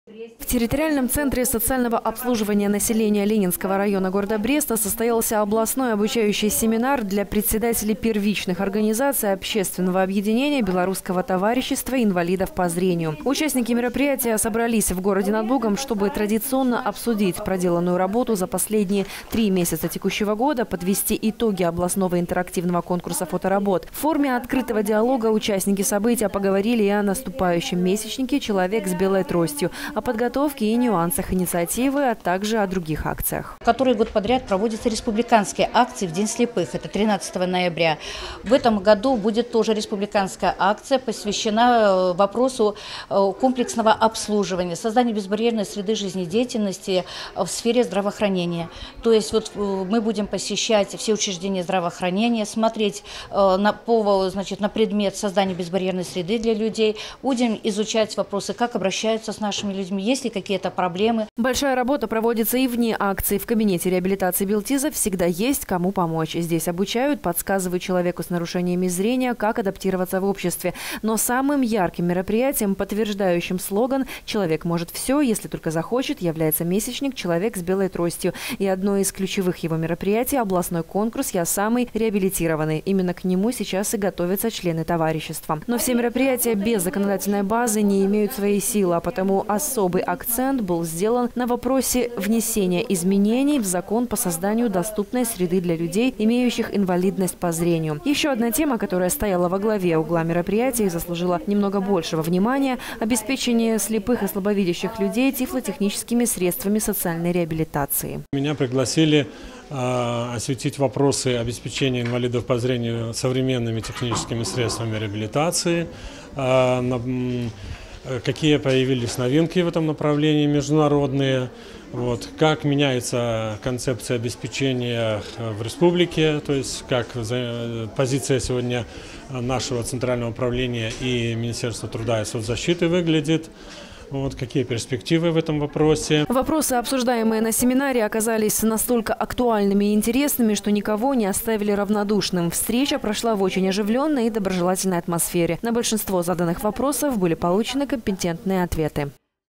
2016-10-04 г. Брест. Семинар в БелТИЗ. Новости на Буг-ТВ.
Найти решение социальных проблем инвалидов по зрению, создать безбарьерную среду для их жизнедеятельности и обеспечить нужные условия для реабилитации и интеграции в общество, повысить уровень и качество их жизни - именно такие задачи ставят перед собой представители белорусского товарищества инвалидов по зрению. Накануне в городе над Бугом состоялся очередной рабочий семинар брестской организаци. Продолжение темы – в следующем материале программы.